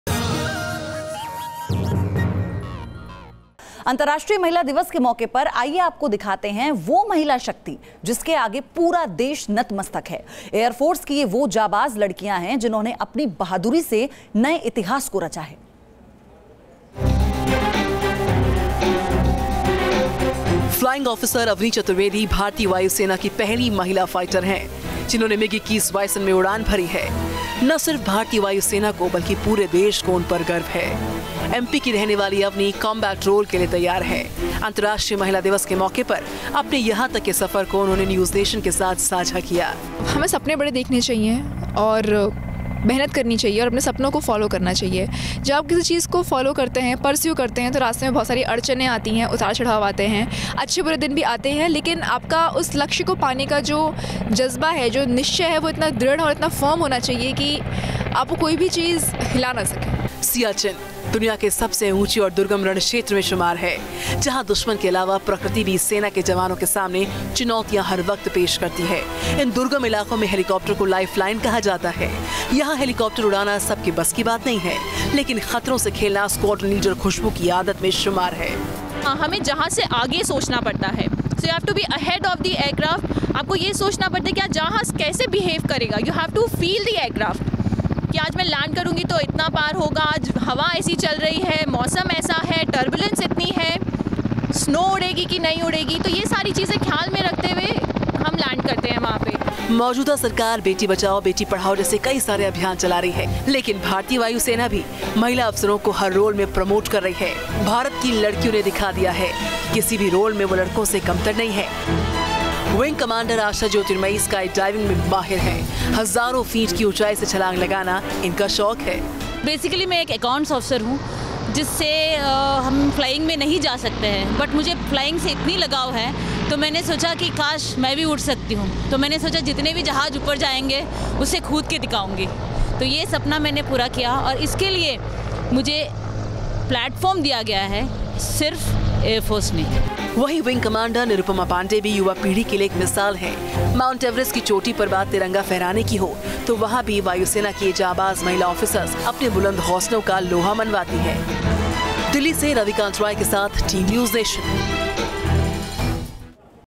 Avani Chaturvedi, India’s first female fighter pilot
Indian Air Force's flying officer Avani Chaturvedi on Thursday became the first Indian woman to fly a fighter aircraft, an IAF official said. "Chaturvedi became the first Indian woman to fly a fighter aircraft solo when she flew a MiG-21 bison in her first solo flight," the official